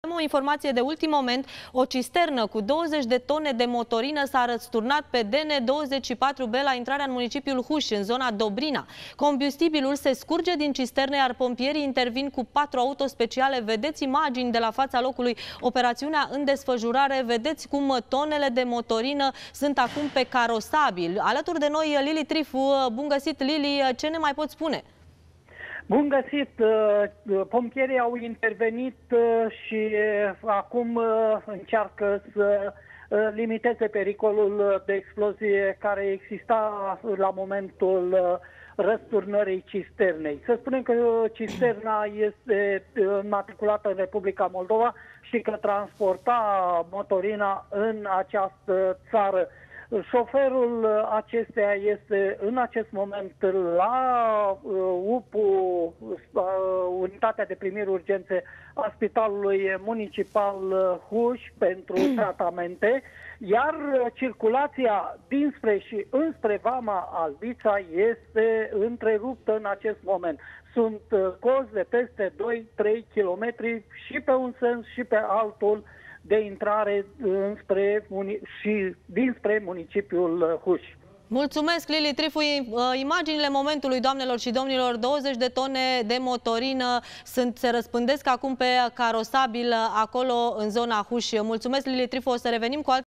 Pericol de explozie! Cisternă încărcată cu 20 de tone de motorină, răsturnată pe un drum judeţean
O cisternă încărcată cu motorină s-a răsturnat astăzi pe un drum județean din Vaslui la intrarea în municipiul Huși, în zona Dobrina.